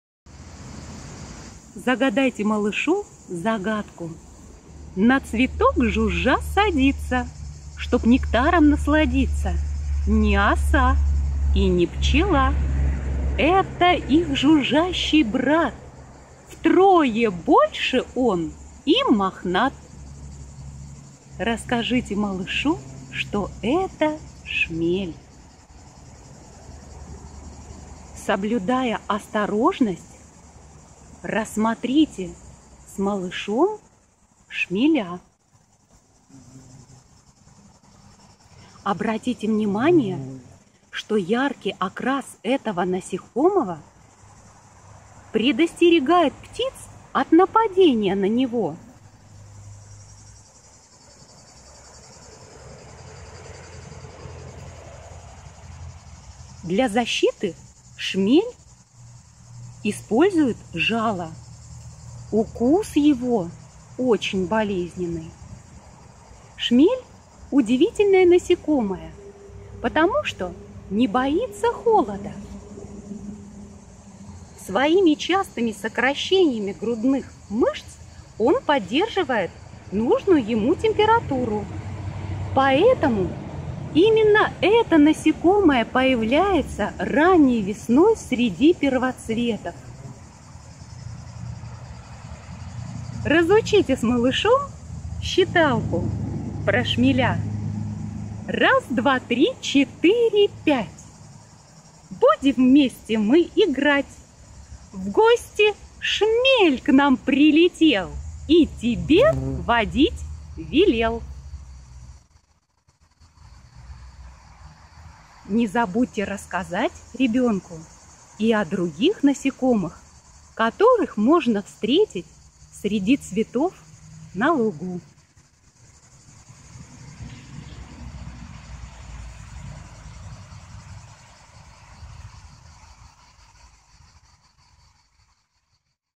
Детям о насекомых. Шмель
Расскажите детям об окружающей природе! Например о шмелях и пчелах, за основу рассказа можно взять наше видео!